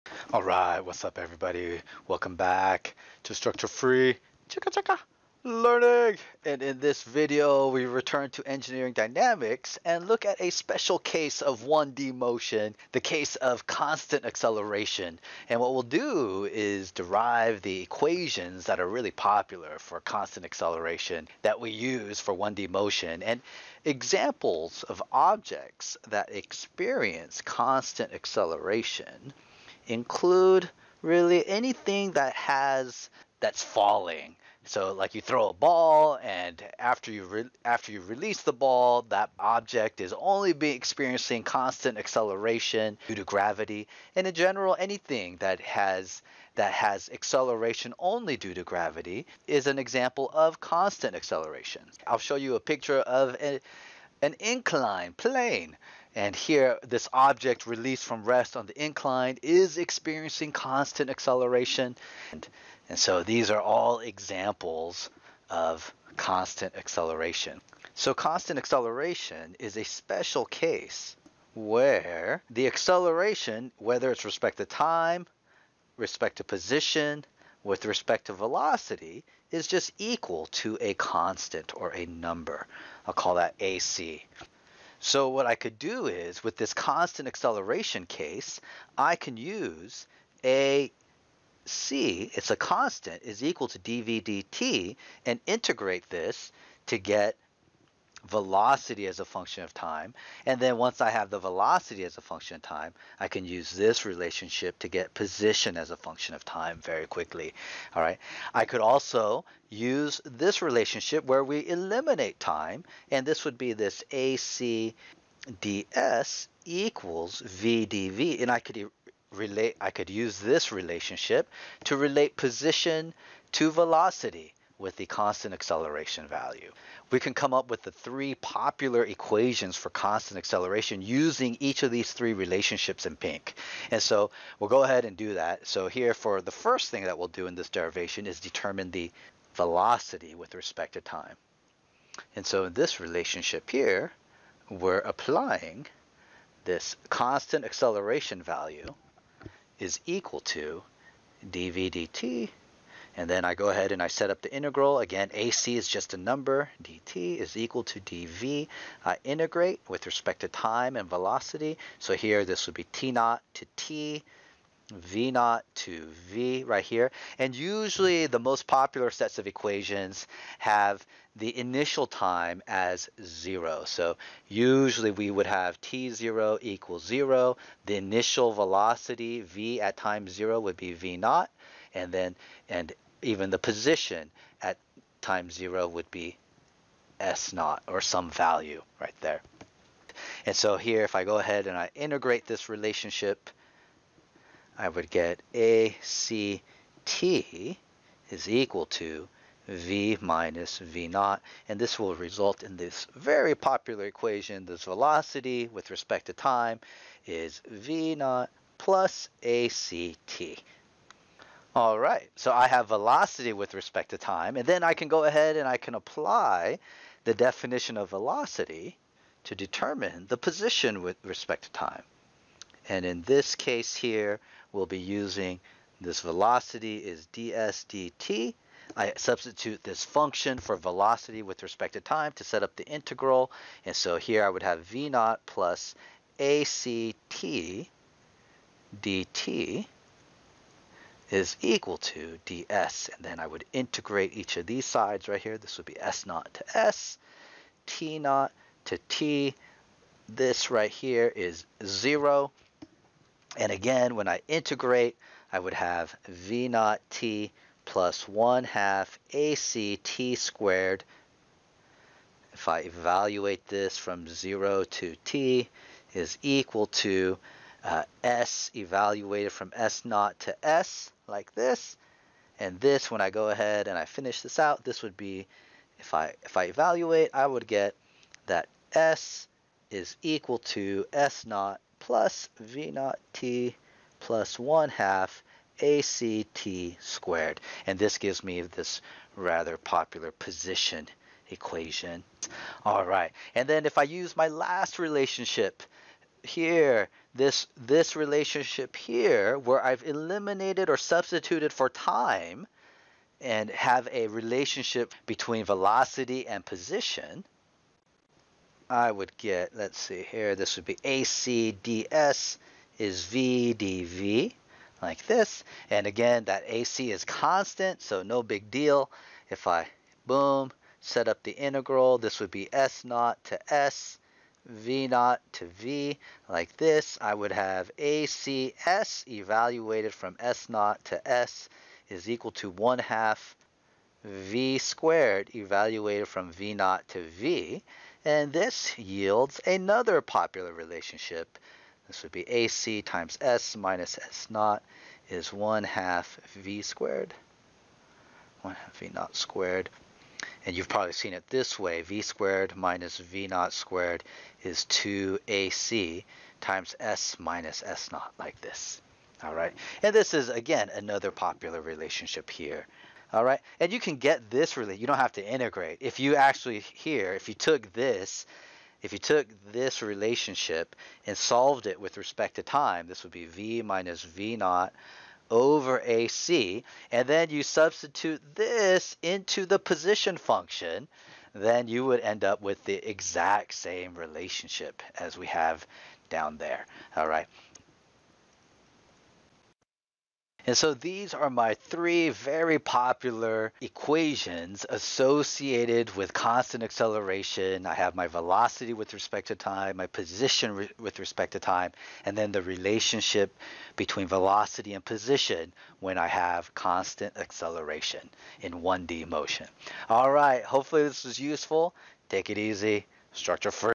Introduction to Constant Acceleration in 1-D Motion - Engineering Dynamics
This video provides an introduction to the special case of constant acceleration and explains where the three popular equations associated with constant acceleration come from.